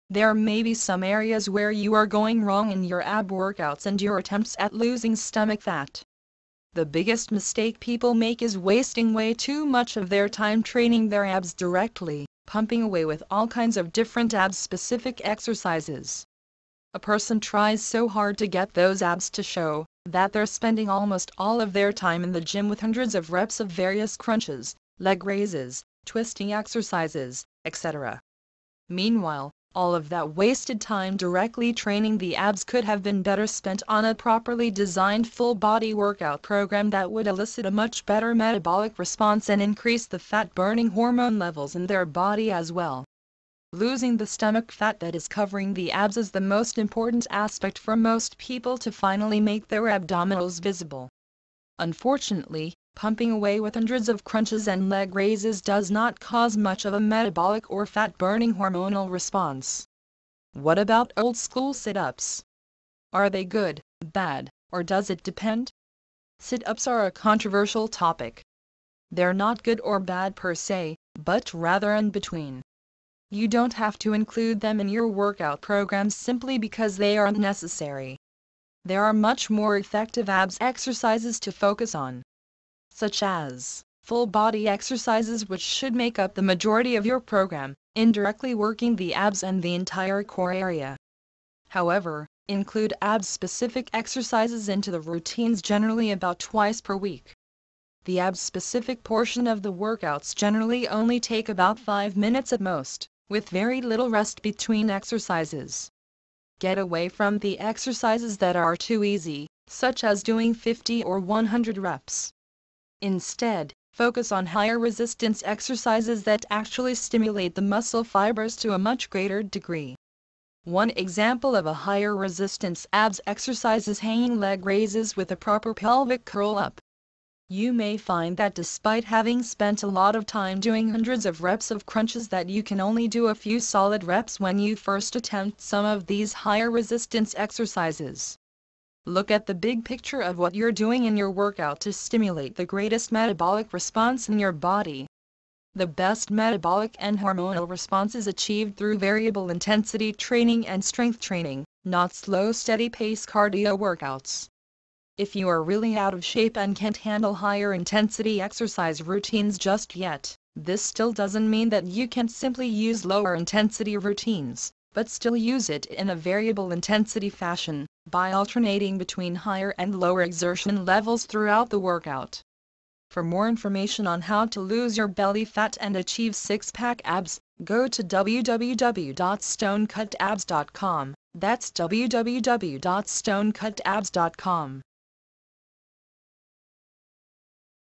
Avoid These Ab Workout And Fat Loss Mistakes
There may be some areas where you are going wrong in your ab workouts and your attempts at losing stomach fat.  Find out why.